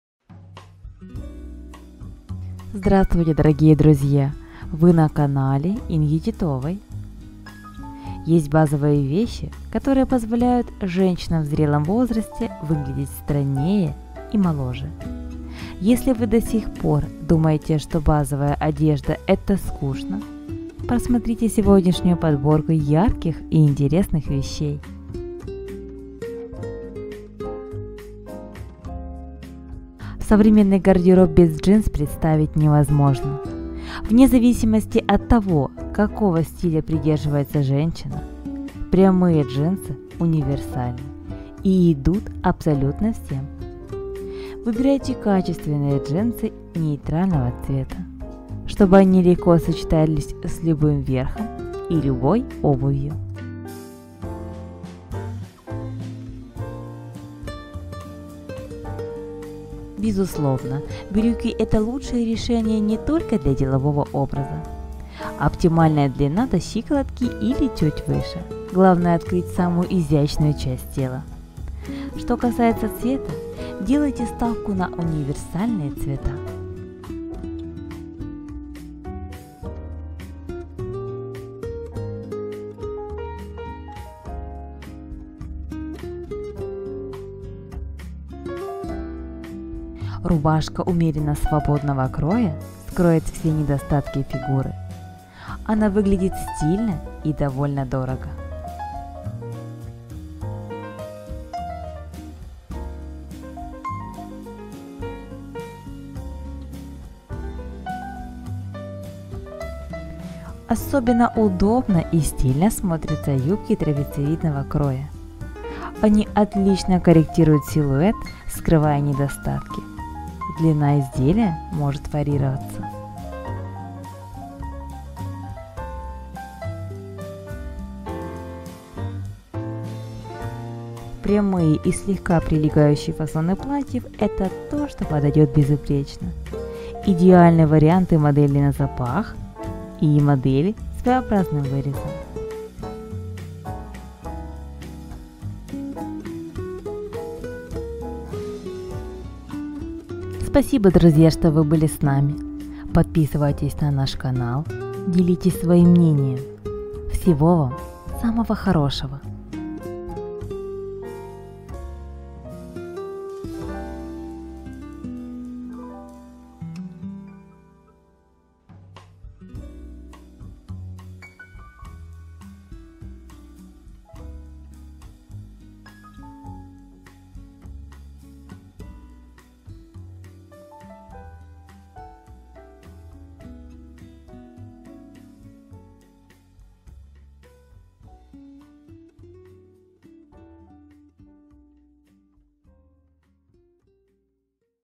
СТИЛЬНЫЕ ВЕЩИ ДЛЯ ЖЕНЩИН ЗА 50
Мода для женщин 50 лет на лето 2021 не привязана к одному стилю. Дизайнеры предлагают разные стилевые решения, а не только привычную большинству классику.
Моя жизнь, творчество, вдохновение.
Дорогие и прекрасные мои зрители, буду очень рада дружить и общаться с вами!